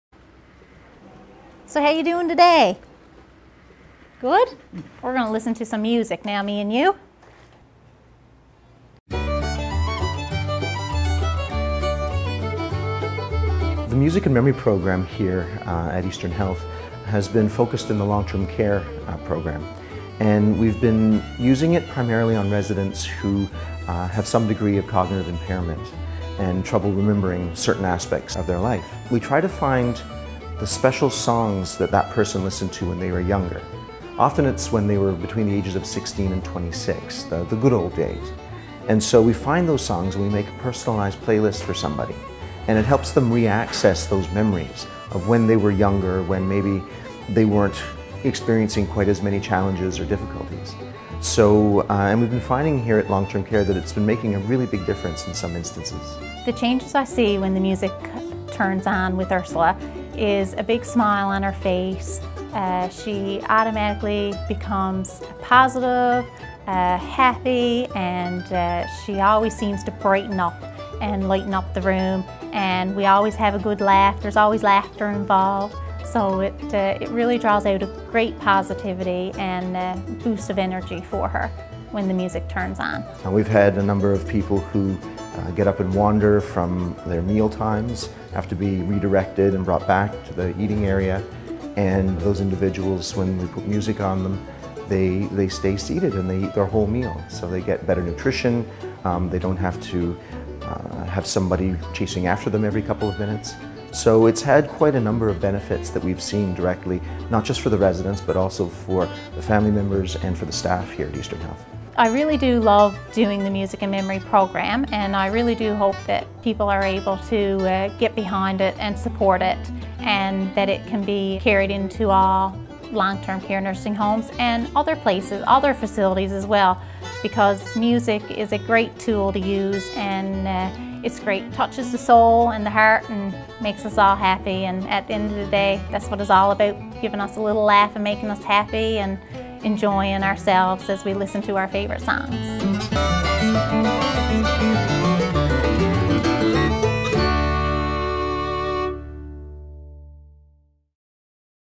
Supporting Joyful Listening
A song has the power to bring us back to a time, a place, an emotion. It can ignite vibrant memories – even for those who are coping with dementia or other cognitive challenges. This is what the Joyful Listening program (previously named Music & Memory) is all about.

Joyful Listening is a therapeutic program that brings music into the lives of seniors and other long-term care residents through the use of iPods with personalized playlists. The program is based on the fact that human brains are hard-wired to connect music with long-term memory. Music that is tailored to personal histories and favourite styles can trigger well-preserved memories, enabling the listener to reconnect with the world.